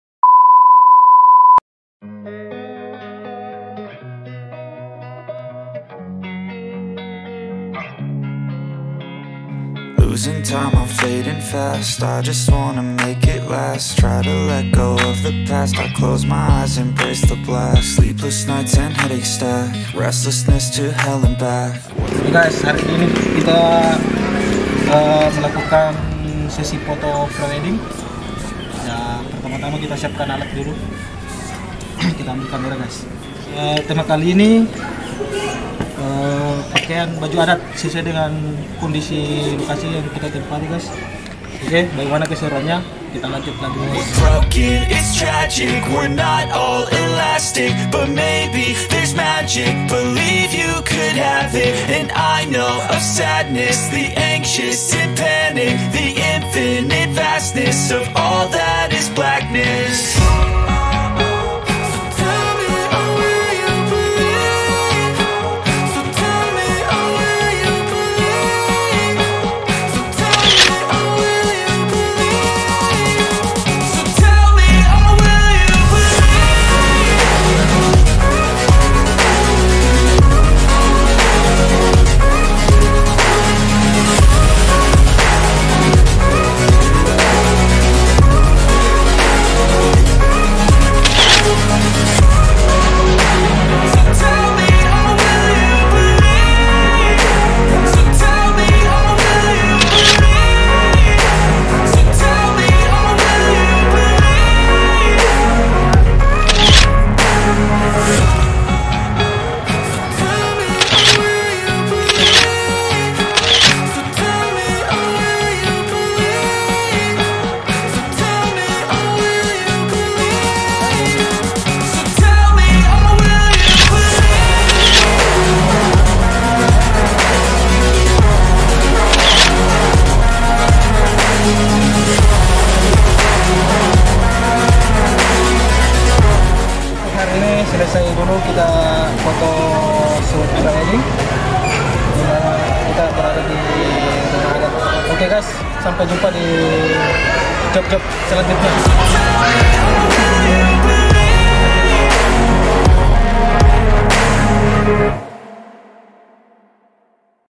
Foto Prewedding Bambang dengan Yusnita | Taken by ardi studio
foto prewedding dengan balutan pakaian Bugis mandar yang di ambil dari rumah adat buttu ciping tinambung dengan kedua pasangan bambang dan Yusnita yang dimana di foto oleh Ardi studio

semoga terhibur dengan videonya
tetap dukung chanel ini dengan cara